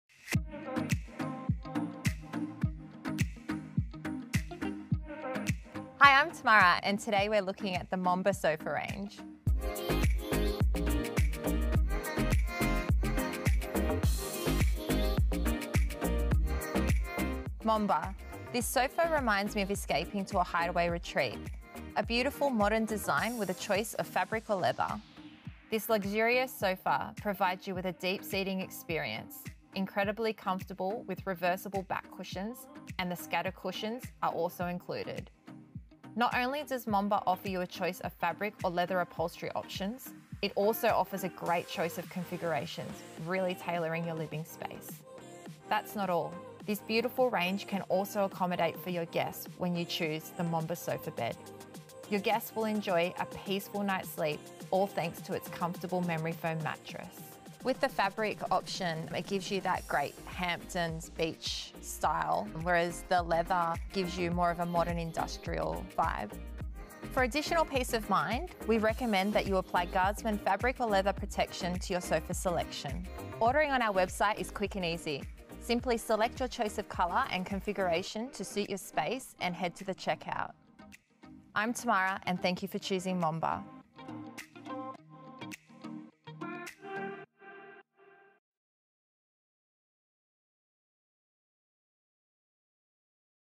Introducing the Momba sofa range
A smart and contemporary design with equally beautiful fabric or leather options, the Momba will suit all styles and homes.